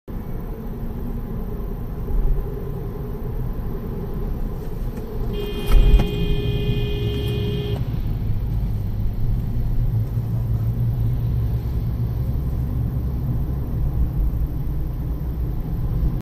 車CAM直擊 - SE5637 307 城巴連環切線 危險駕駛
SE5637 29/06/2023 Time 00:10
網友提供